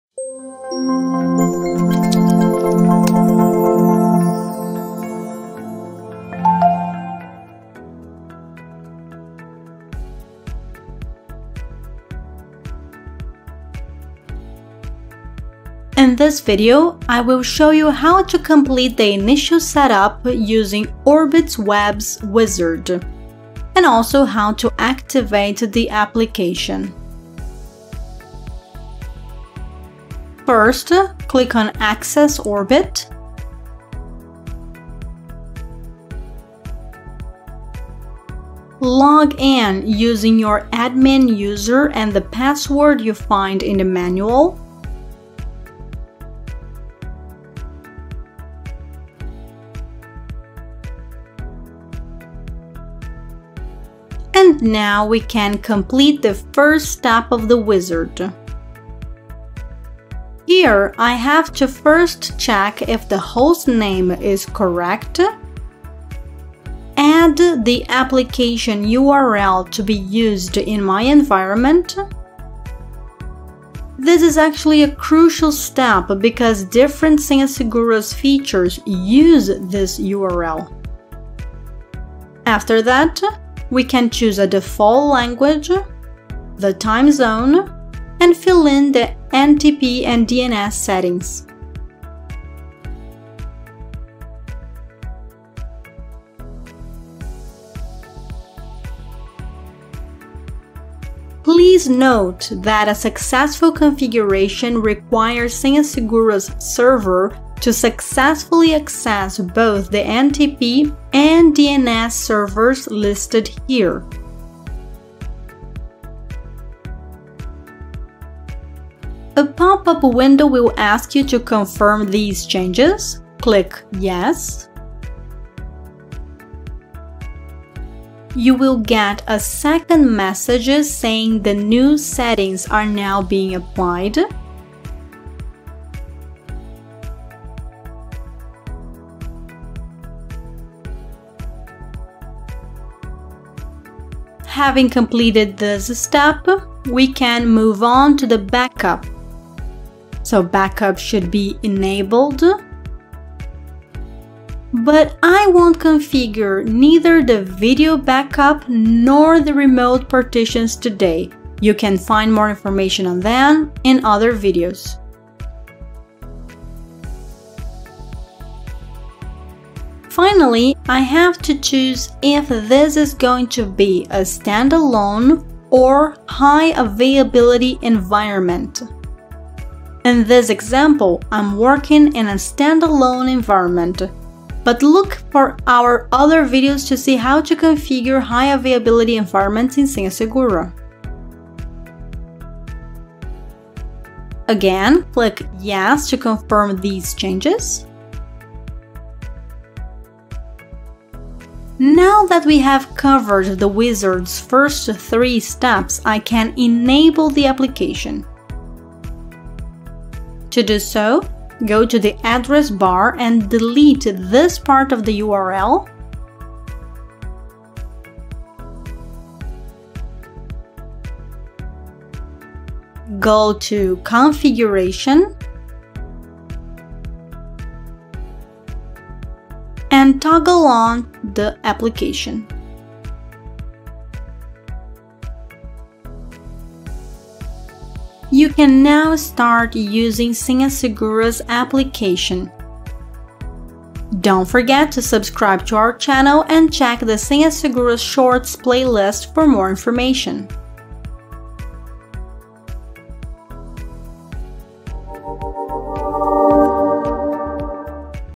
How to setup Wizard and activating the application
In this video, you will check how to set up Wizard and activate the application:

1. Click on the ‘Access Orbit’ button, will be required to log in with the system default user.
2. On the ‘Server settings’ screen enter the hostname, application URL, application title, language and locale settings, timezone, NTP settings, DNS settings, domain, and search information.
3. Click on the ‘Next’ button and confirm the action.
4. On the ‘Backup’ screen select Yes to ‘I would like to enable backup application’  and in “I would like to activate the backup session files?’ and ‘I would like to mount a remote partition?’ select ‘No’.
5. Click on the ‘Next’ button.
6. On the ‘High Availability and Contingency’  select ‘No’ to ‘Do you would like to enable high availability and/or Contingency environment?.
7. Click on the ‘Finish’ button and confirm the action.
8. Delete the /sync/view parameters from the URL and access it.
9. Access ‘Configuration’ and activate the flag ‘Enable application’ and then click on ‘Save’.